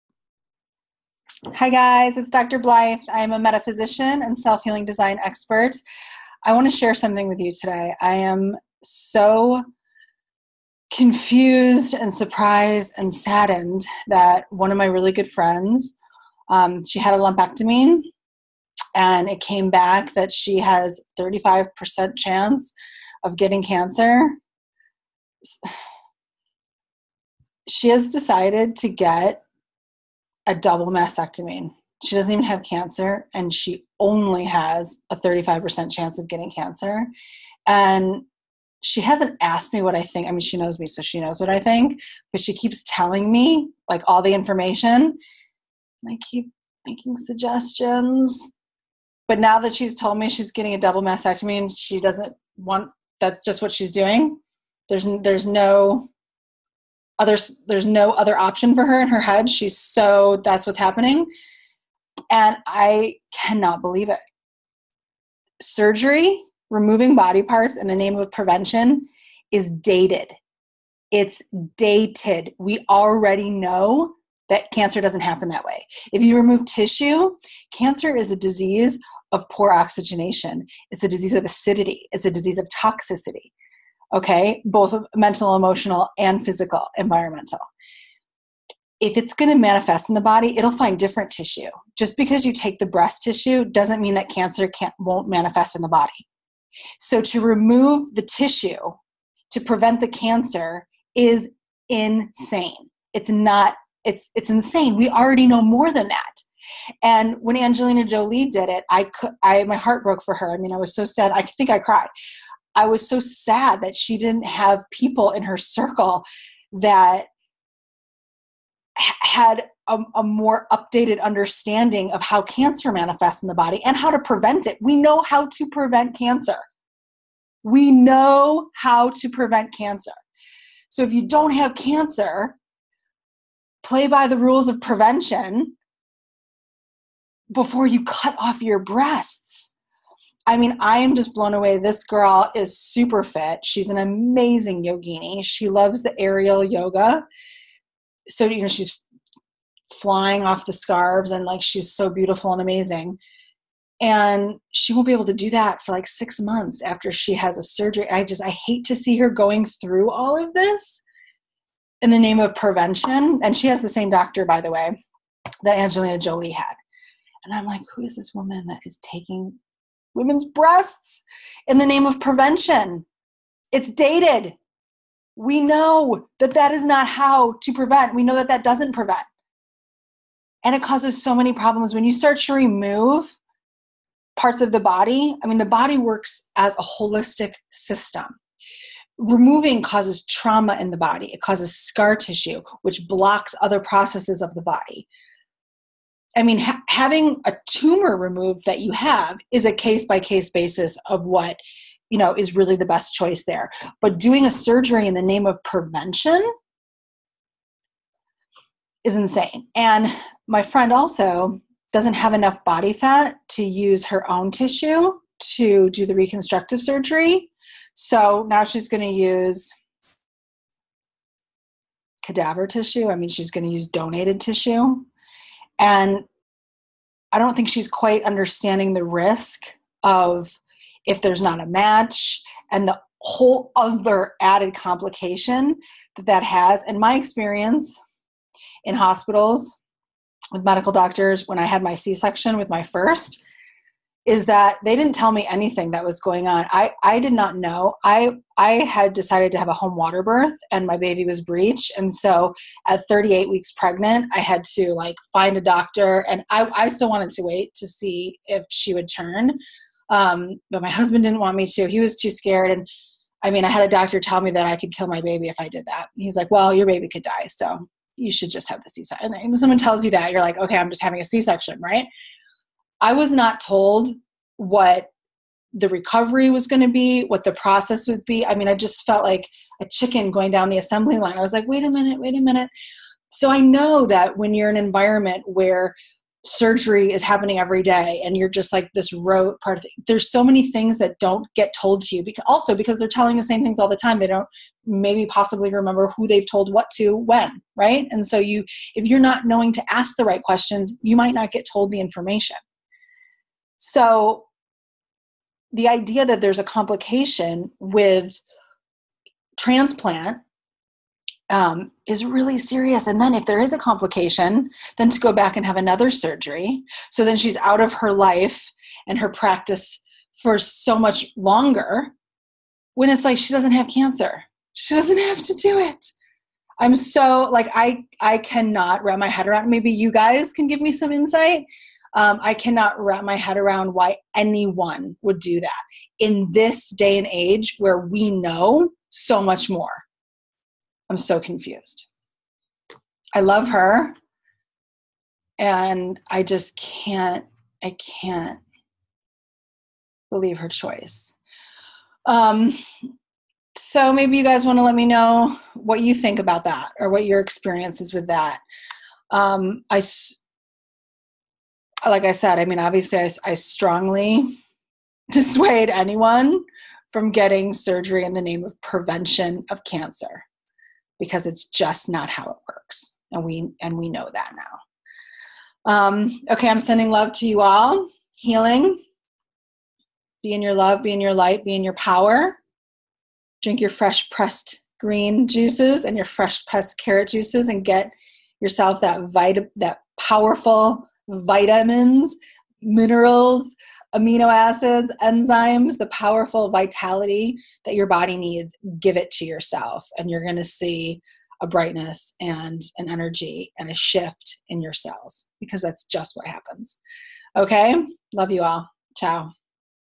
Cancer, Surgery as prevention rant
Surgery as prevention is dated. Watch this video to learn why surgery is not an acceptable approach for prevention of cancer.